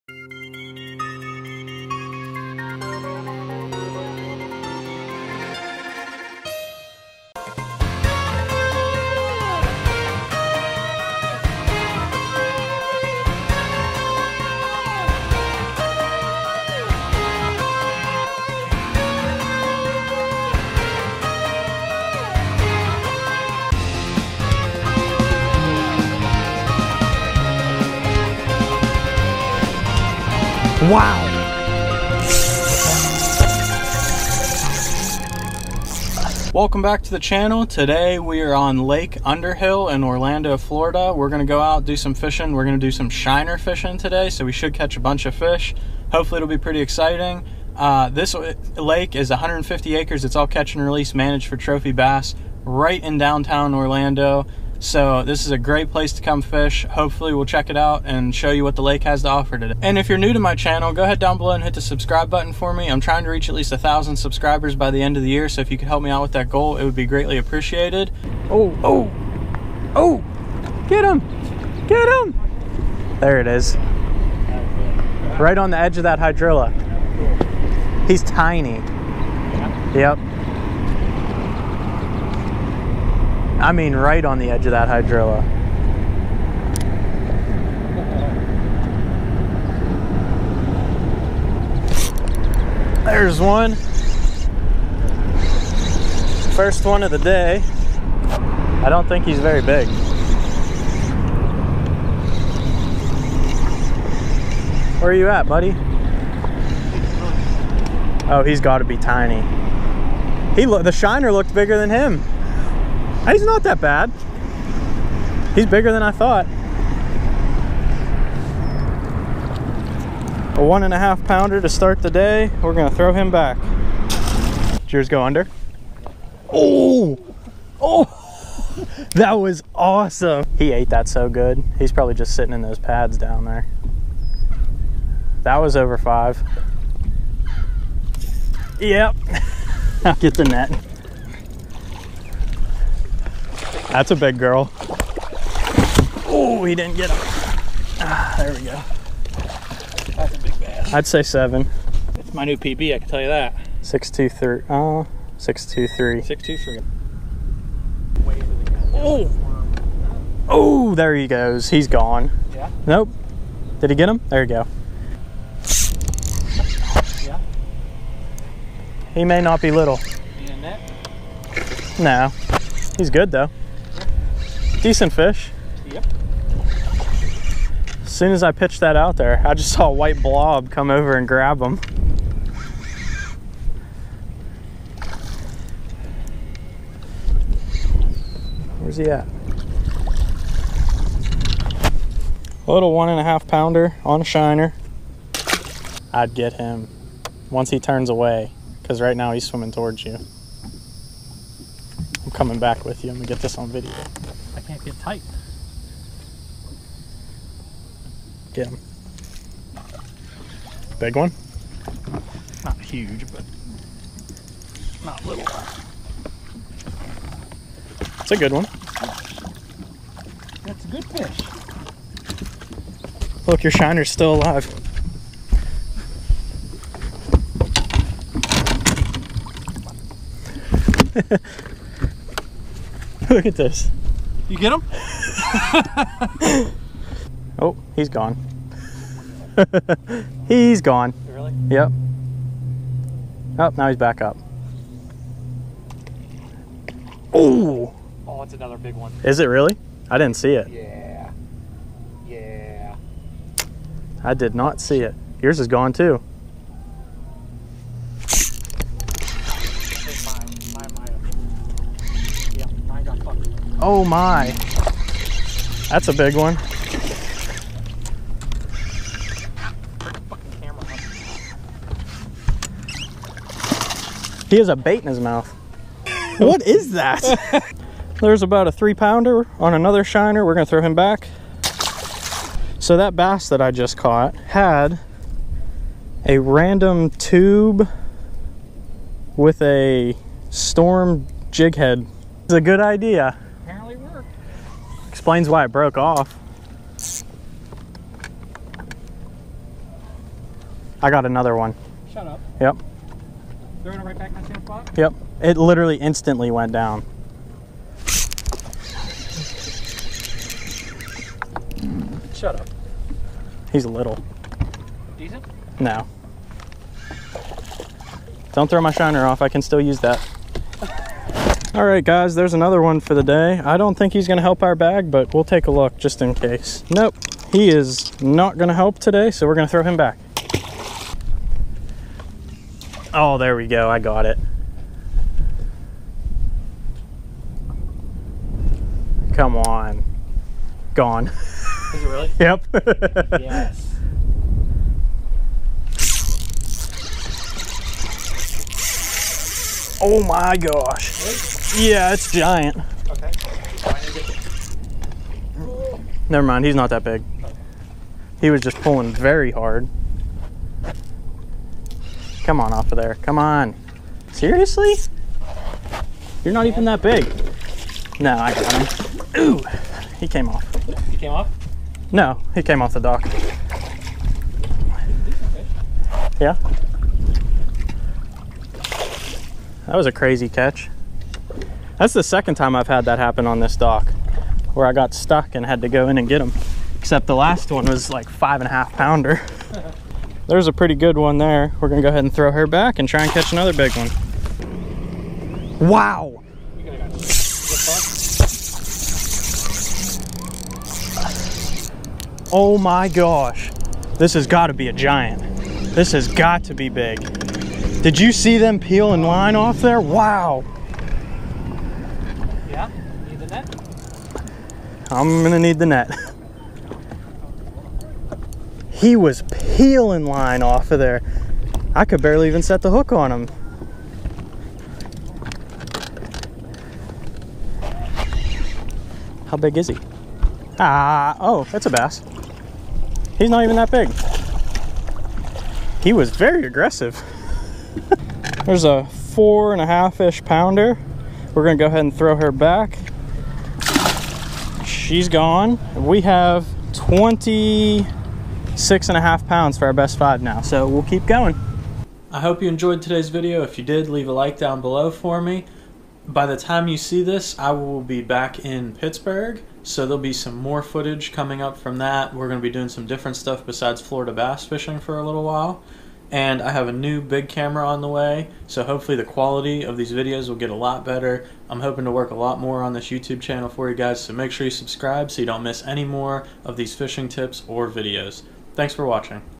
Fishing with GIANT Shiners for BIG FLORIDA BASS! - Shiner Fishing for Bass
We went fishing with GIANT shiners for BIG FLORIDA BASS! Shiner fishing for bass is such a fun time and really easy to get people involved in fishing! Fishing with golden shiners in Florida is a popular way to catch some Big Florida bass if not the biggest bass of your life. Fishing with shiners for bass is such an easy way to catch a ton of fish as well! I hope you enjoy the video of us fishing with live shiners and catching a bunch of Florida bass!